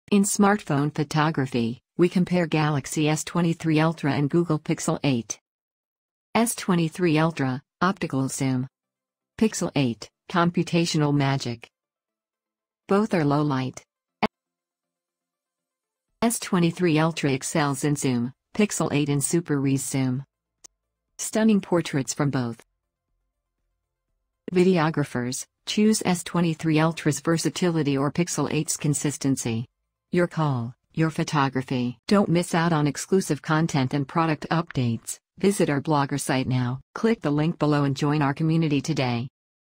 samsung galaxy s23 - samsung galaxy s23 ultra unboxing - Samartphone4
Welcome to our in-depth review of the Samsung Galaxy S23 Ultra! In this video, we'll dive into this incredible device for our U.S. viewers. In the ever-evolving world of smartphone photography, we compare the Samsung Galaxy S23 Ultra and the Google Pixel 8 to find out which is the ultimate camera phone. The S23 Ultra features a versatile quad-camera setup with optical zoom, while the Pixel 8 relies on the magic of computed photography. Both excel in low light, with the Pixel 8 often turning night into day. The zoom capabilities favor the S23 Ultra's optical lens, but the Pixel 8's Super Res Zoom also impresses. Both create stunning portraits with bokeh backgrounds and showcase the software's advancements. Videographers face a tough choice as both offer impressive video capabilities. In the end, it comes down to personal preference: S23 Ultra for versatility, Pixel 8 for consistent excellence. Choose your camera phone based on your photography priorities.